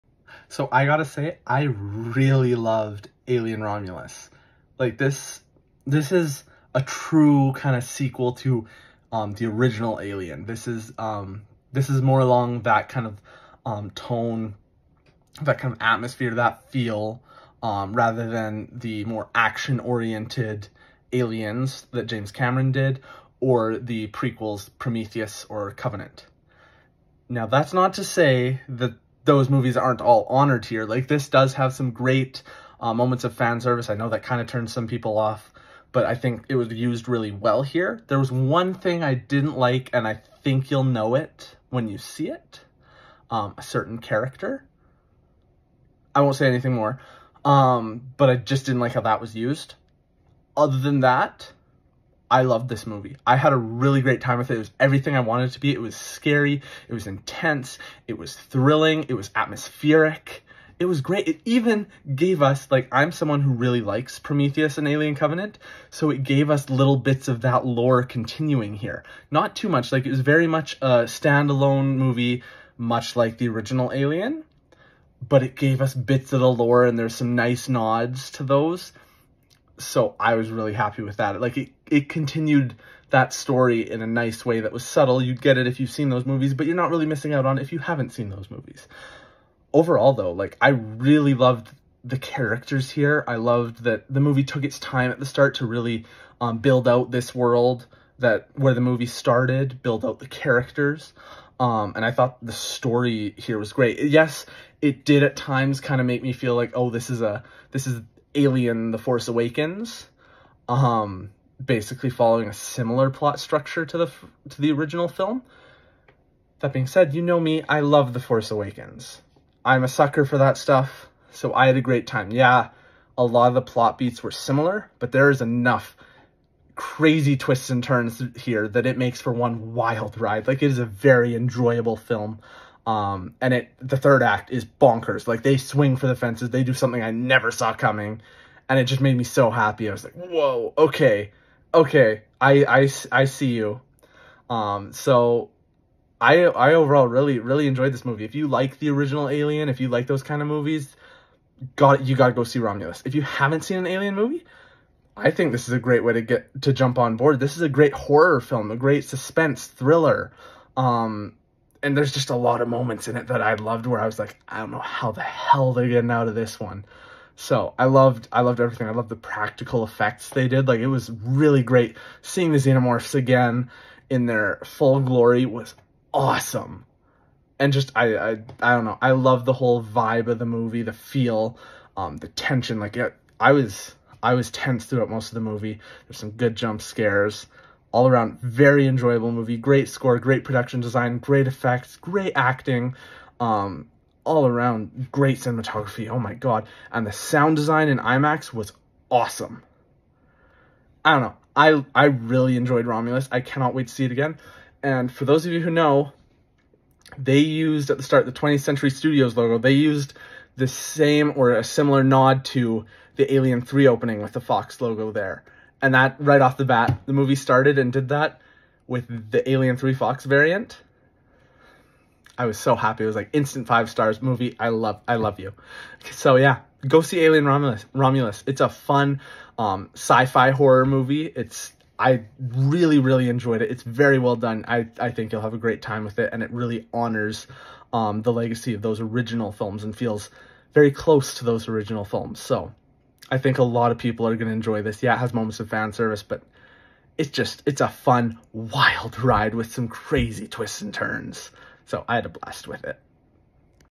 Alien: Romulus - Quick Review
My first thoughts right after seeing the newest entry in the Alien Anthology: Alien Romulus!

While scavenging the deep ends of a derelict space station, a group of young space colonizers come face to face with the most terrifying life form in the universe.